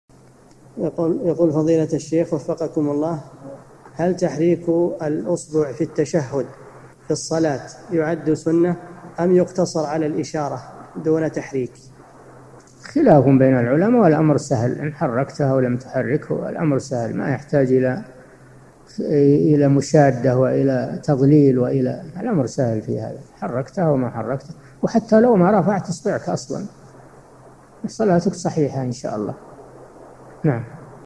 [1325 /1350] هل تحريك الأصبع في التشهد في الصلاة يعد سنة أم يقتصر على الإشارة دون تحريك؟
-[1325 /1350] السؤال: هل تحريك الإصبع في التشهد في الصلاة يعد سنةً أم يقتصر على الإشارة دون تحريكٍ؟
الجواب: خلافٌ بين العلماء والأمر سهل إن حرَّكته ولم تحركه والأمر سهل ما يحتاج إلى مشادة وإلى تضليل، الأمر سهل في هذا حركته أو ما حركته، وحتى ولو وما رفعت إصْبِعك أصلاً صلاتك صحيحة إن شاء الله.
للاشتراك في قناة الشيخ صالح الفوزان
أسئلة وأجوبة من شرح قاعدة جليلة ( 1350 ) مقطع

للاستفسار والإقتراحات والدعم
رئيس الفريق
لمشروع كبار العلماء